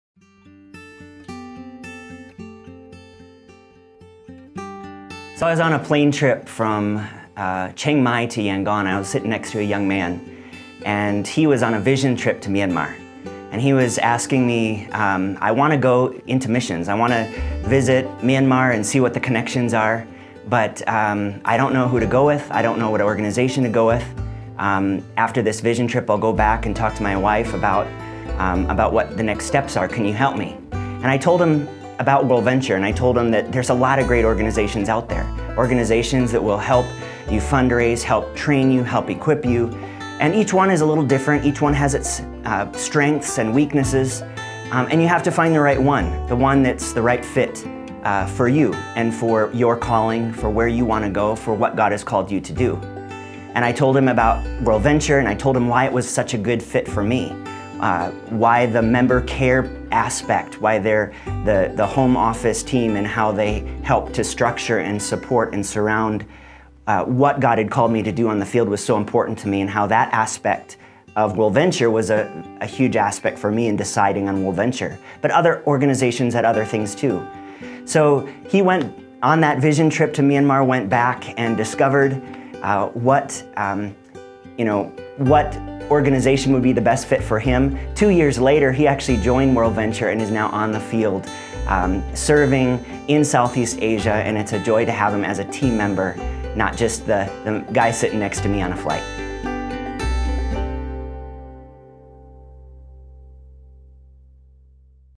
More Than A Guy On A Flight
Hear this 1-minute video of how Jonathan met a future Global Worker to Asia on a plane.